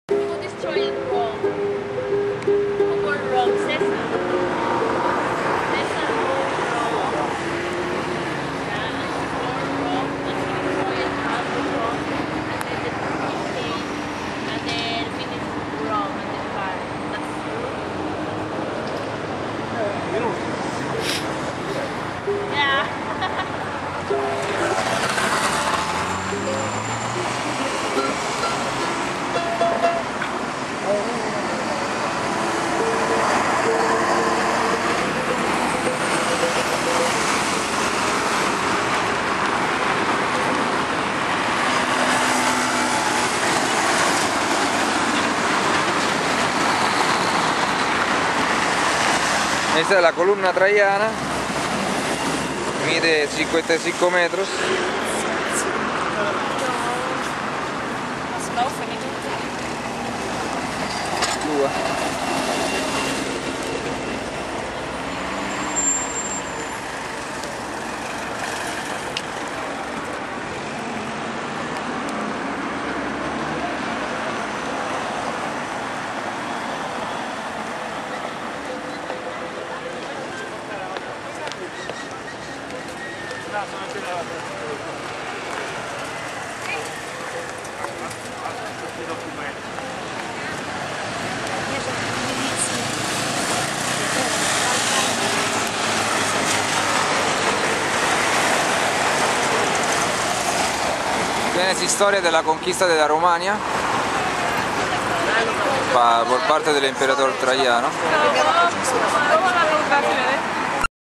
Descubriendo Roma con Giacomo Sanesi. 038 - foro romano
Videos dedicados a mis aficionados amigos suramericanos que no tienen la posibilidad de llegar en Roma y ver mi maravillosa ciudad.
Paz,
Giacomo